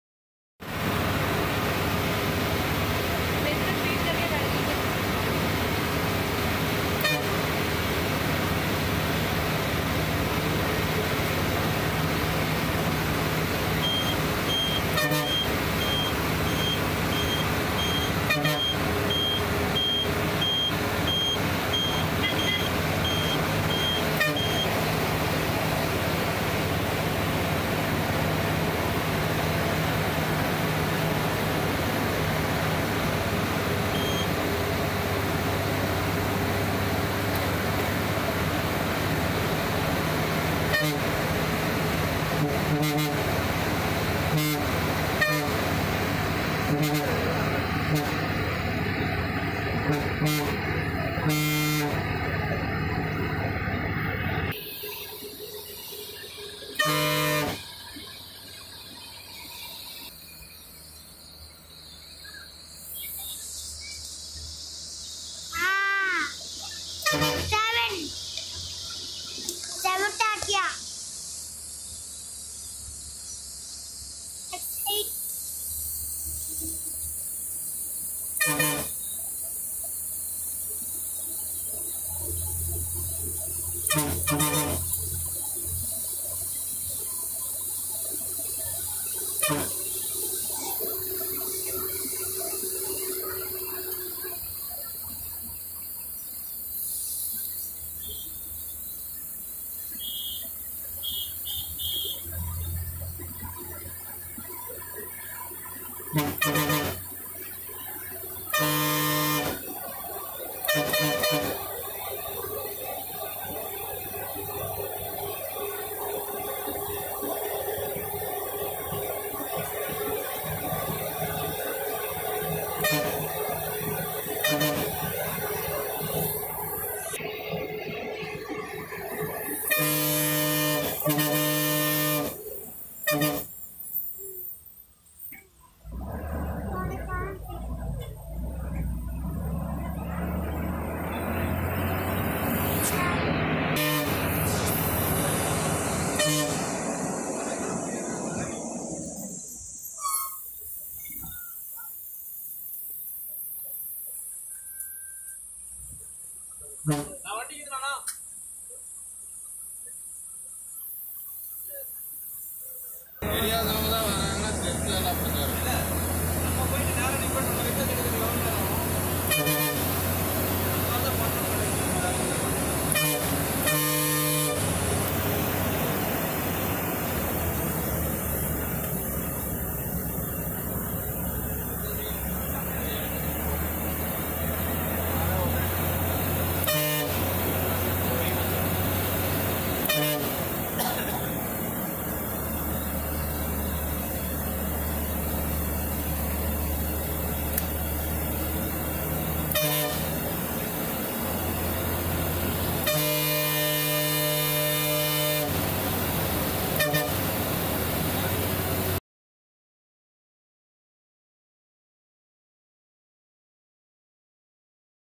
BUS & TRUCK ACCIDENT AVERTED - Hairpin Bends GHAT ROAD [Salem To Yercaud]
Bus accident averted.  Hairpin Bends GHAT ROAD [Salem Yercaud By Bus]
The Salem Yercaud Ghat road has 20 Hairpin curves on a 22 KM plus of stretch. A close call at 2.28 in video when the bus and truck came pretty close on a sharp curve.
TamilNadu State Transport Buses are available every half an hour at day time.Road is well built till yercaud.
Yercaud is a hill station in Salem District, in Tamil Nadu, India. It is located in the Shevaroys range of hills in the Eastern Ghats.
The town's name is derived from the lake located at its center – in Tamil Yeri means lake and Kaadu means forest.